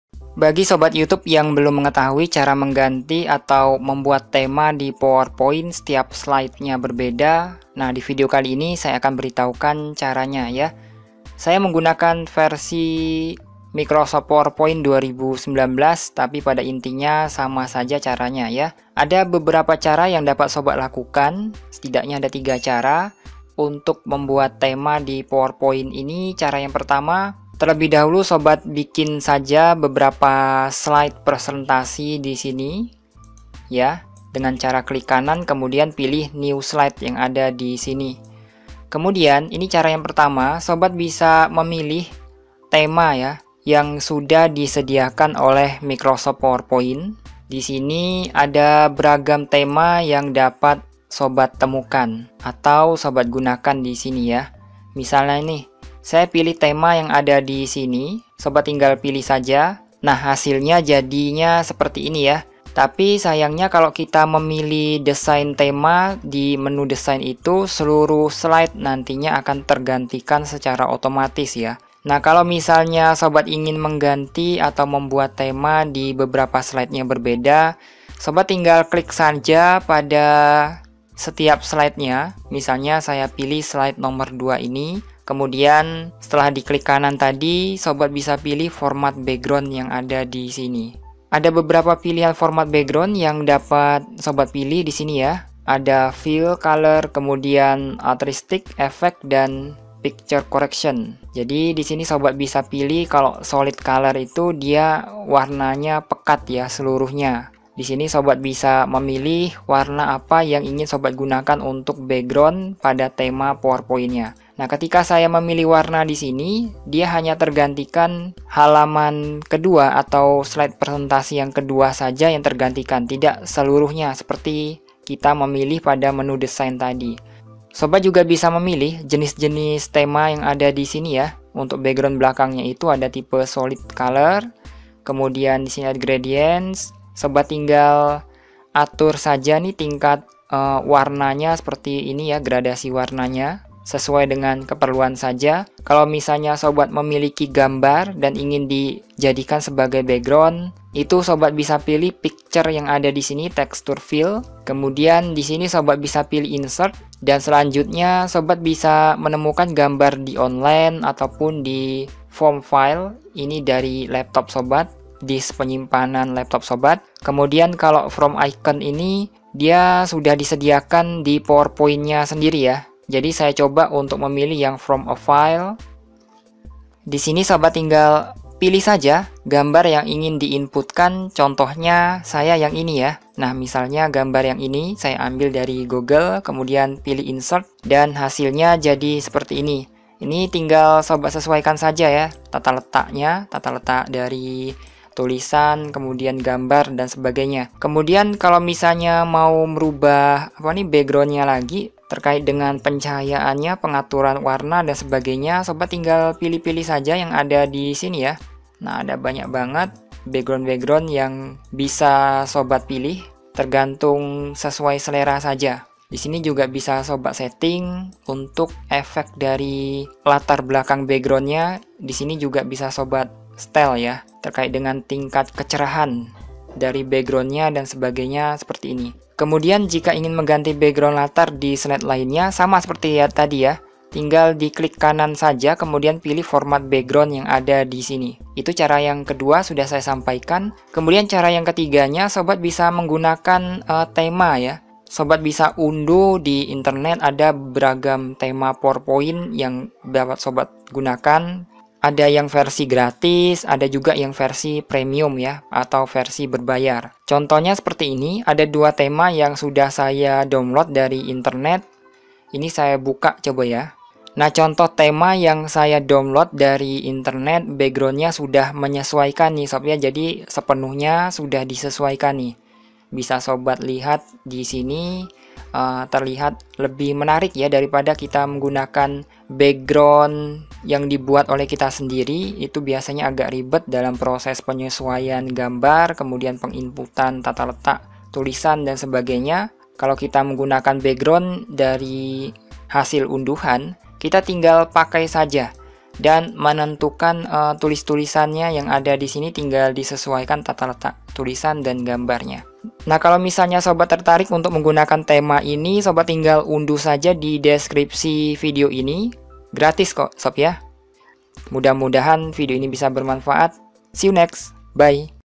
Cara Membuat Background Powerpoint Berbeda Setiap Halaman
Untuk membuat background powerpoint berbeda setiap halaman caranya sangatlah mudah. Ikuti saja panduan video tutorial berikut ini.

Terima kasih atas SUBCRCRIBE, LIKE, KOMENTAR dan SHARE,
Mudah - mudahan sobat diberikan kemudahan rezeki oleh Allah SWT.
Amin...